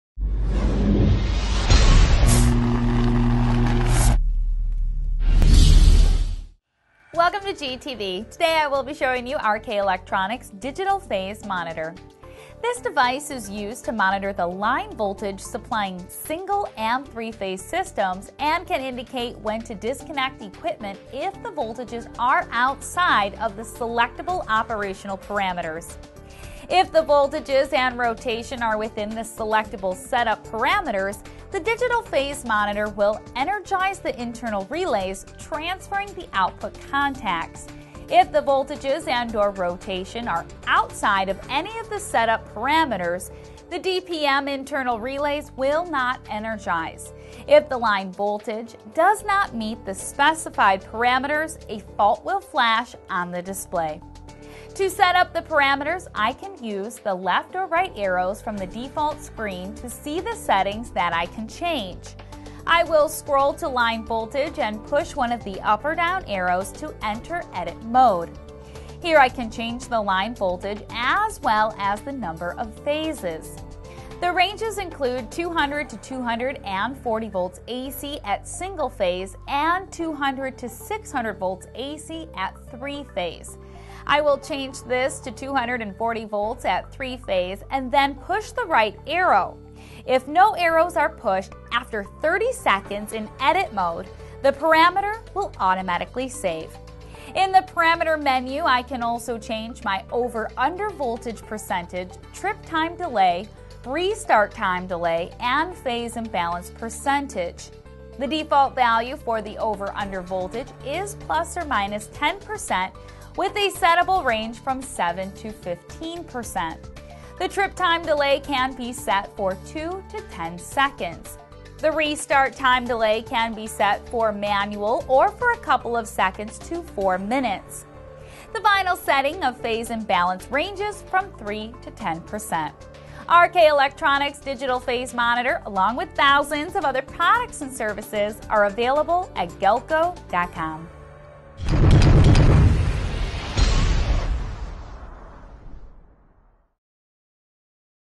RK Electronics Digital Phase Monitor
RK Electronics Digital Phase Monitor presented by Katie Rydzewski for Galco TV.

DPM Series Features:
-    Single & Three Phase
-    200 to 240VAC, Single Phase
-    200 to 600VAC, Three Phase
-    All settings retained if there is a complete loss of power
-    DPM settings can be adjusted while voltage is disconnected

The DPM is a Digital Phase Monitor that will monitor line voltages from 200VAC up to 600VAC. Actual line voltages and set‑up parameters are set via the digital display. The supply voltage must be available during operation.

And for the full line of RK Electronics Products go